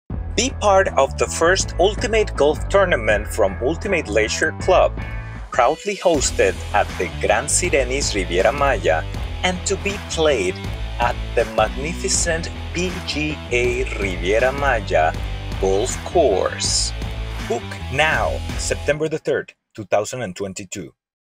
Ultimate Golf Tournament - 1st Edition
Come and join us on September the 3rd for a fantastic tournament experience in a PGA Golf Course and play for the chance to win several prizes, participate in a raffle of all-inclusive free weeks, and a special prize to be announced for the winner of the tournament.
🏌️‍♀️ Sign up and let the fun begin! ✨⛳️